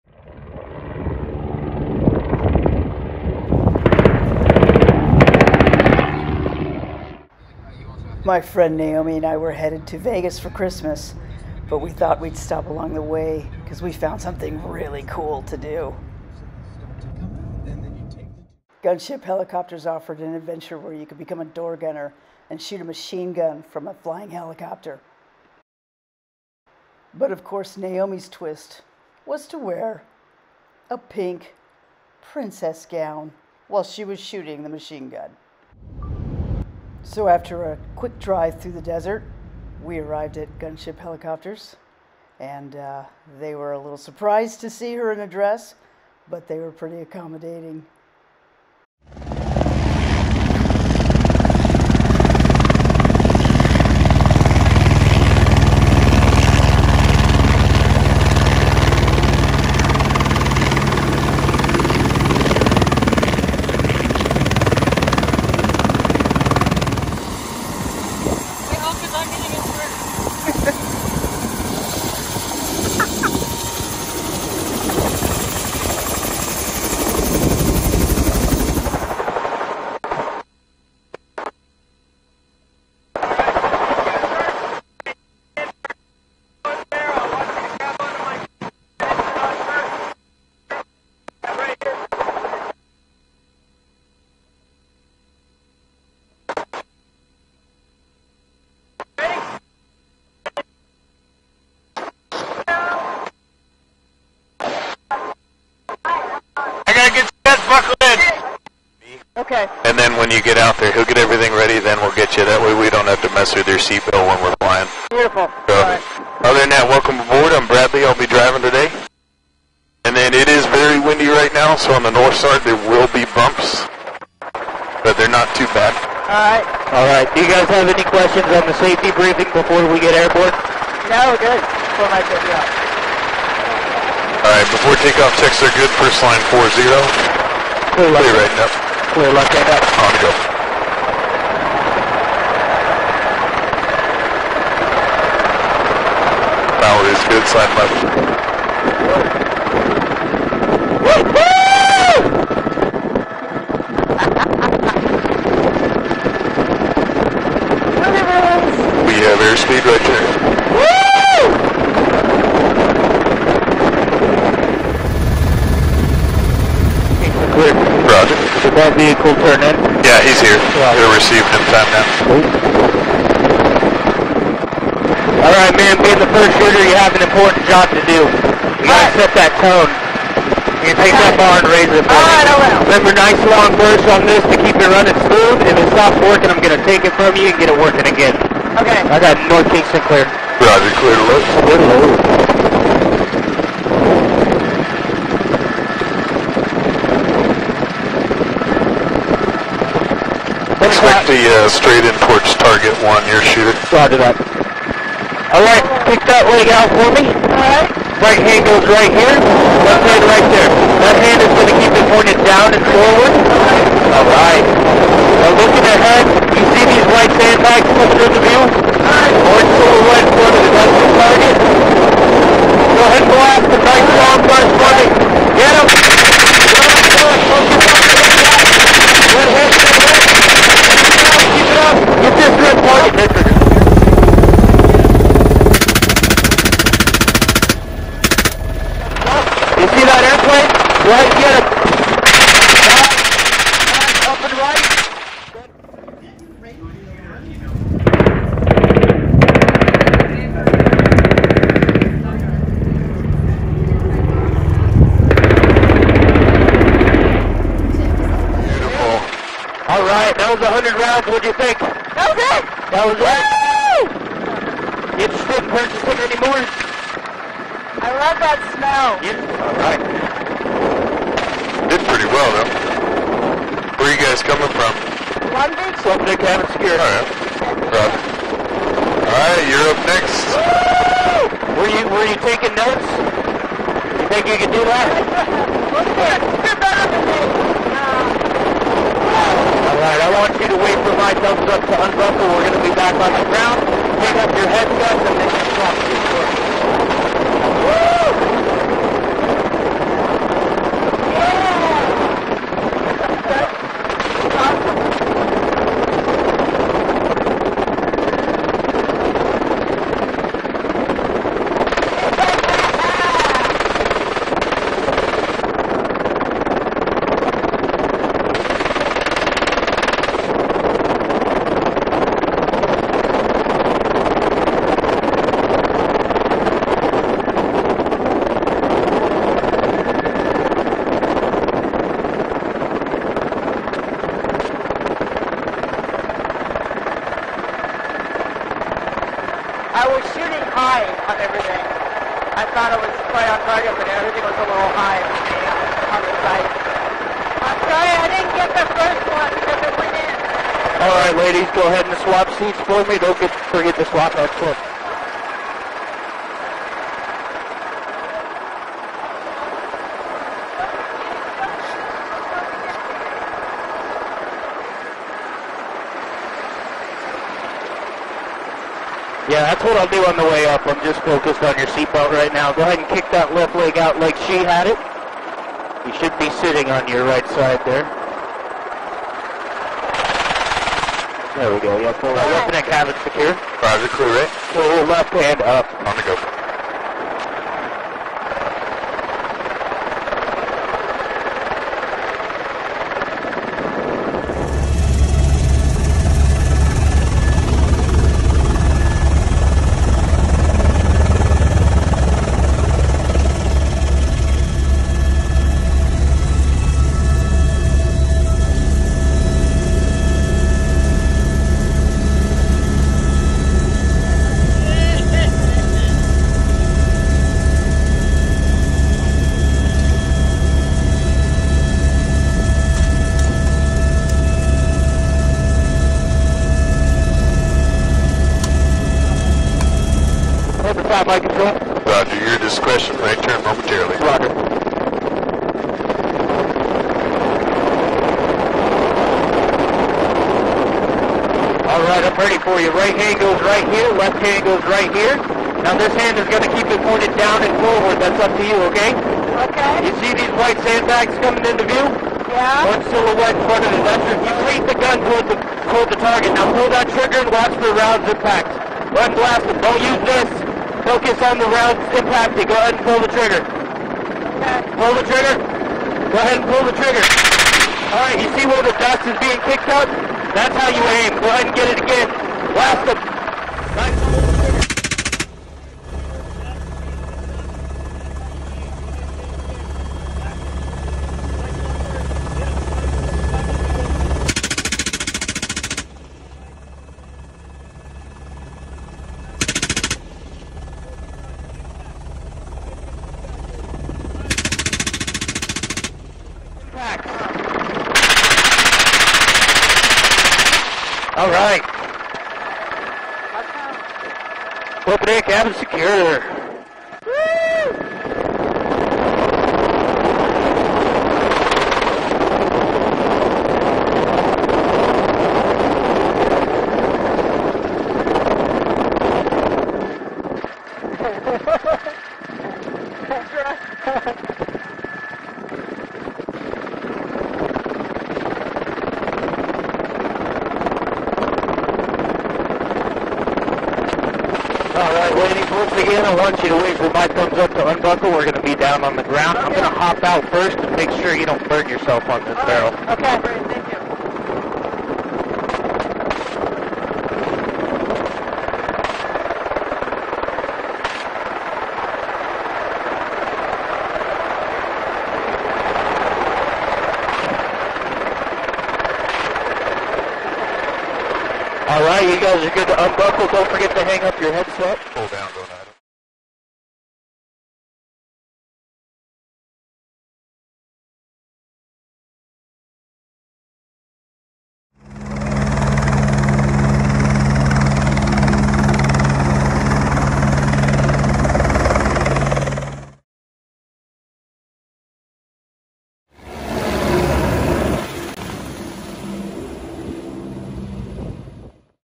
Machine Guns and a Princess Dress SD 480p
On our way to Las Vegas for Christmas fun my friend, Naomi, and I decided that we wanted to shoot machine guns out of a helicopter. Thus, we spent a day as door gunners. with a twist.